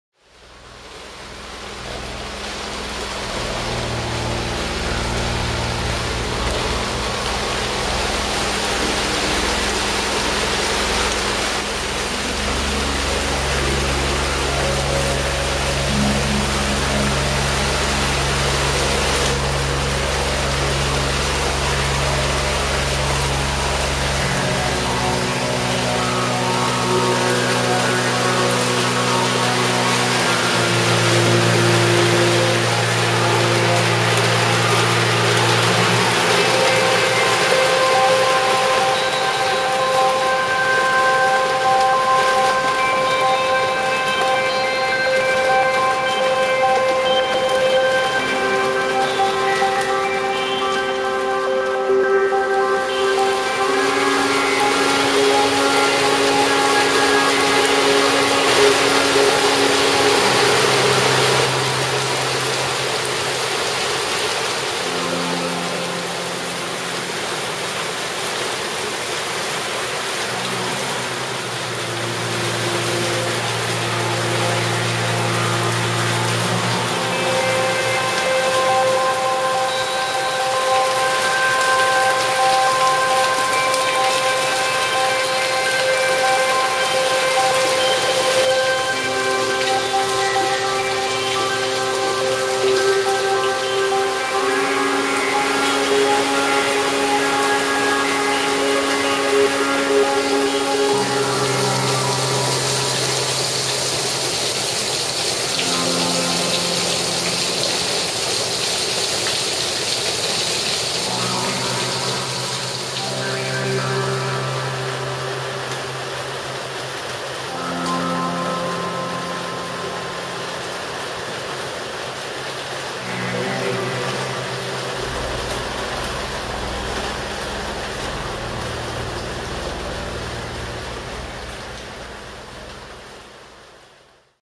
Heavy Rain. Flooded Patio.
June 23, 2011 rain. Queen Street West, Toronto. La Hacienda Patio.